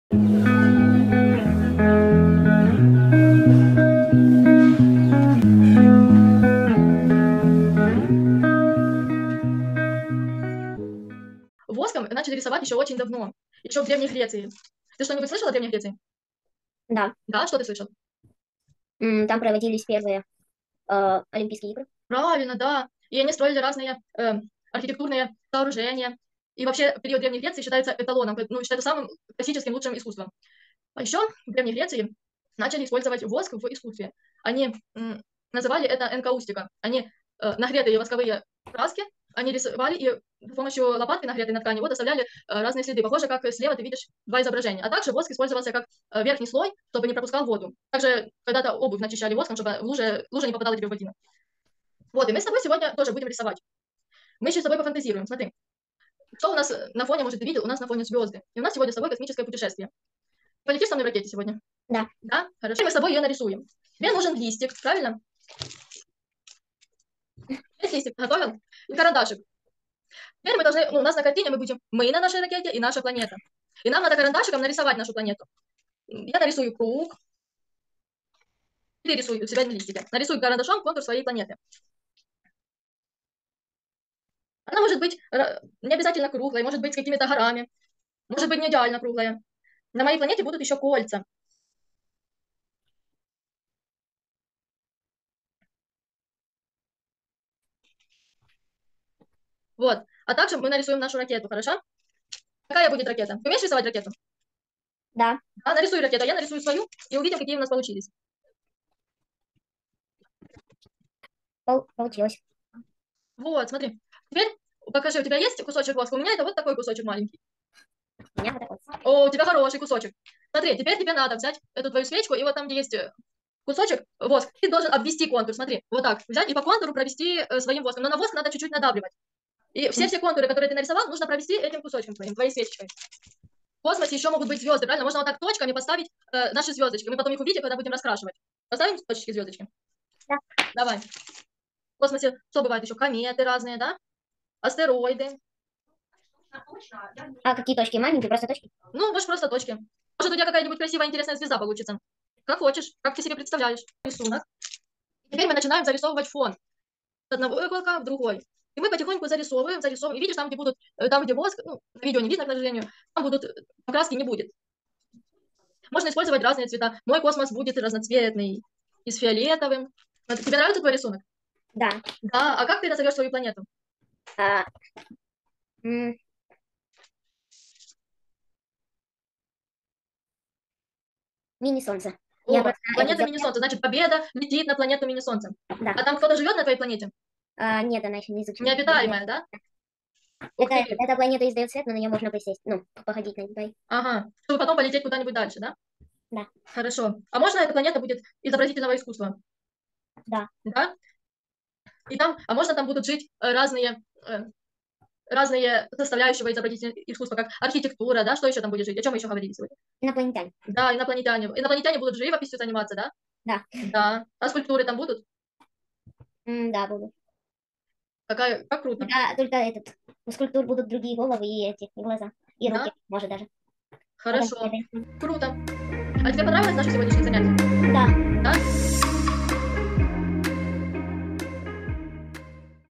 Космическое путешествие в страну изобразительного искусства. Воскография
На занятии мы:
🎨поработали над словарным запасом, узнали новые слова и термины.
🎨Узнали об исполнении воска в рисовании.
🎨развивали логику, память, фантазию.
🎨пробовали применять свои дизайнерские решения.
🎨работали с такими материалами как бумага, краски акварель, кисти, воск(свеча), карандаш.
🎨Исполнили работу: "Моя воображаемая планета" в технике Воскография.